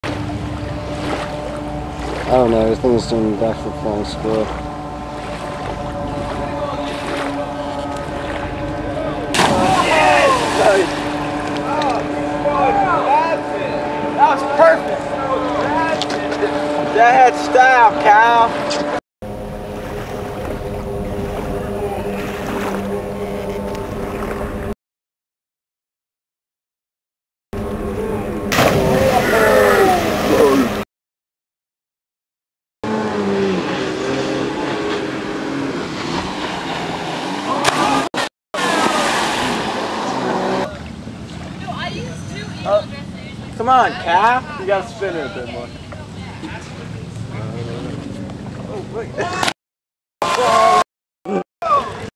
Rope Swing Flying Squirrel Backflip!
My brother Kyle executes a flying squirrel reverse backflip off a rope swing into the Delaware river from Milford, N.J.